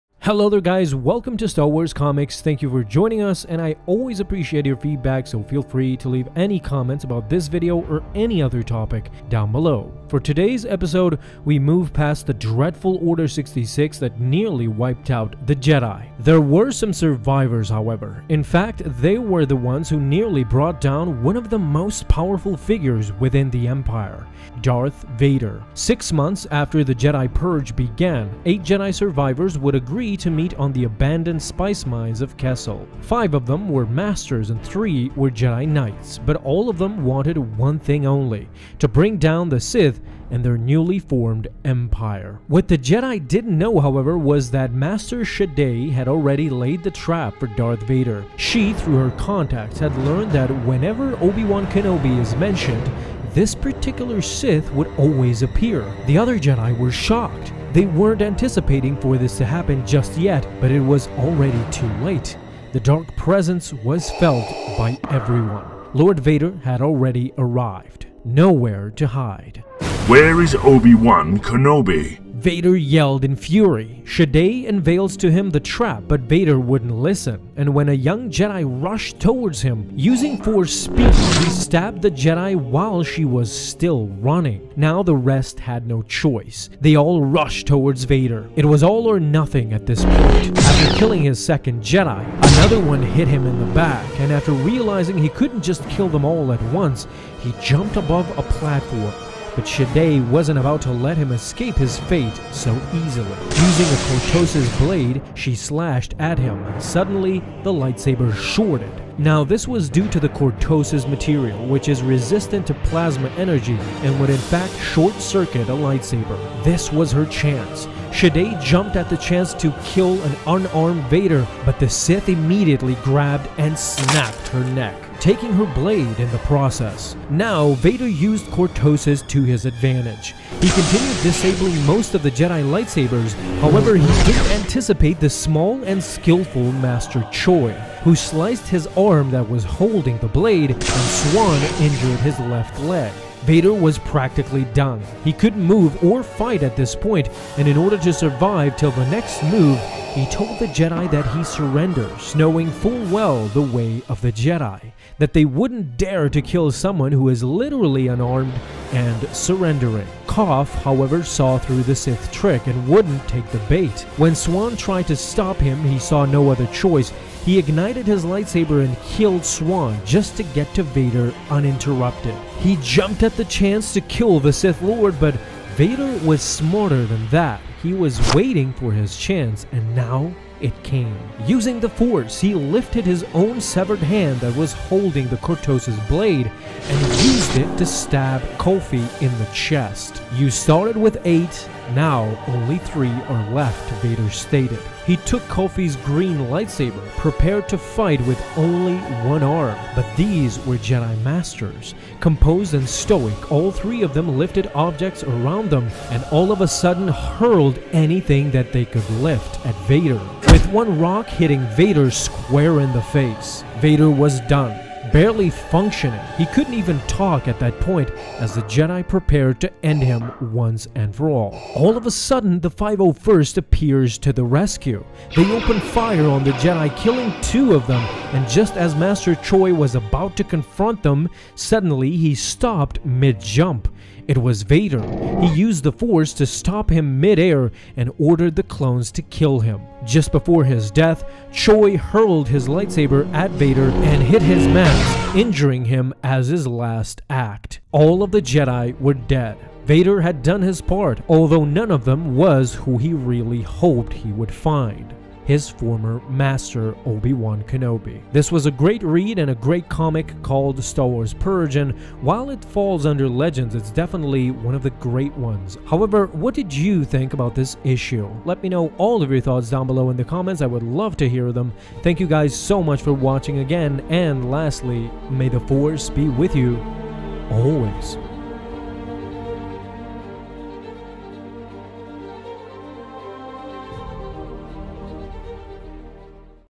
The CLOSEST Darth Vader Came to DEATH After Order 66(Vader Voiced) - Star Wars Comics Explained
Shortly after Order 66, Darth Vader while relentlessly searching for his former master Obi Wan Kenobi and that is what lead him to defeat and close death.

Source:  Star Wars Purge